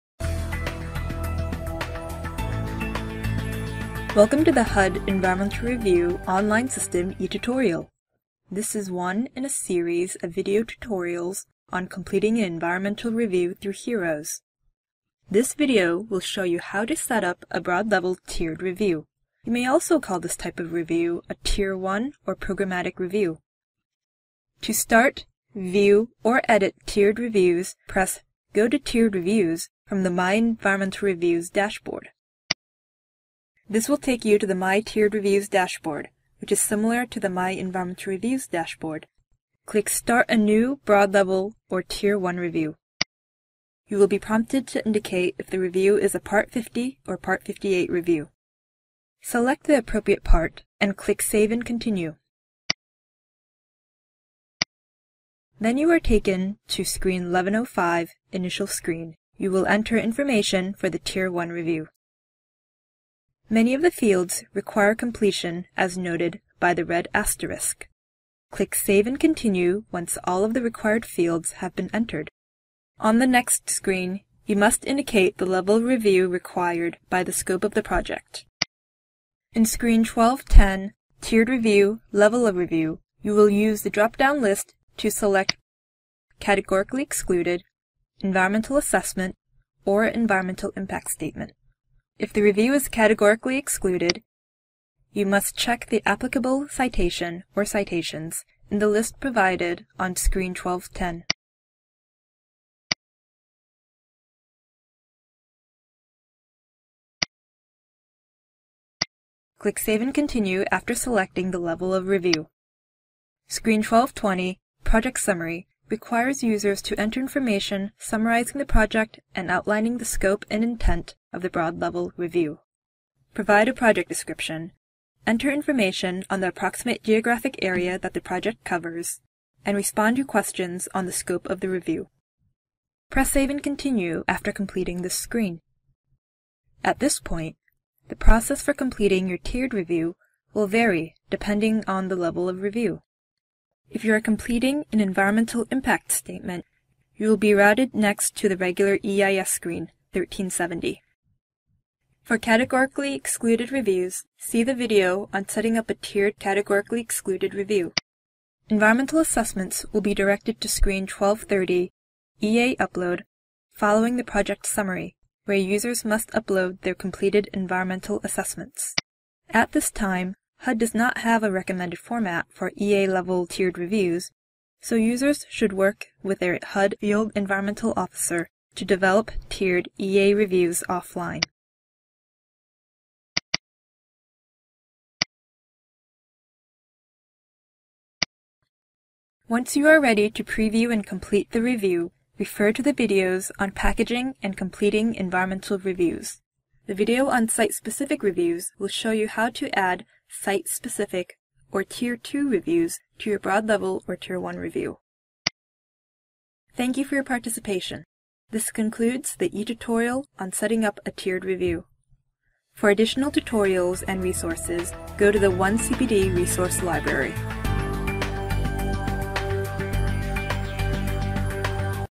HEROS How to Video: Setting up a Broad-Level of Tier 1 Review
This e-tutorial is part of a series of trainings demonstrating the functionality and components of the HUD Environmental Review Online System. This session explains how to set up a broad-level tiered review (also called a tier 1 or programmatic review).

To access related training materials & learn about getting credit via OneCPD Learn, visit OneCPD Training & Events